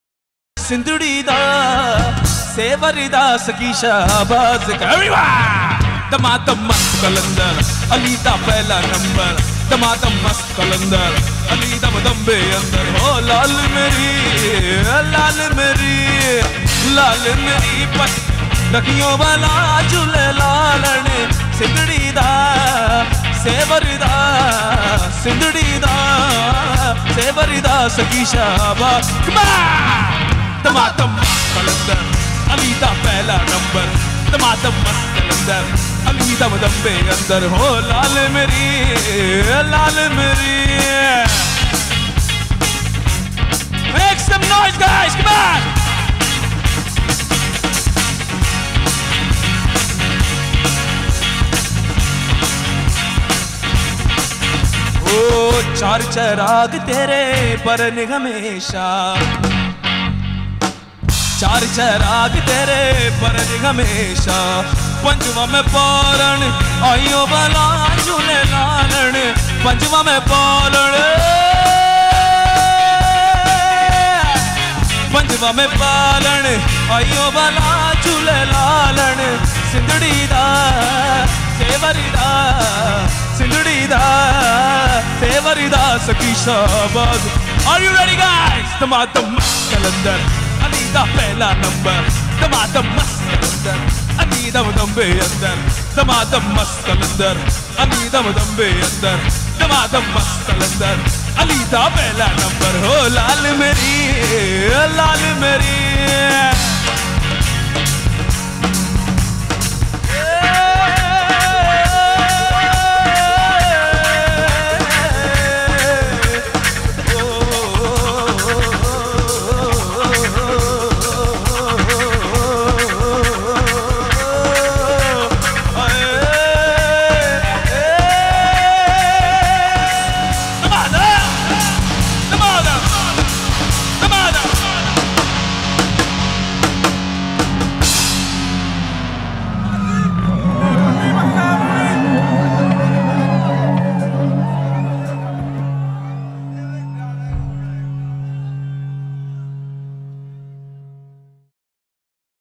GURUZ
live at Hon'ble Prakash singh Baadal 's house ,Baadal village (near bhatinda)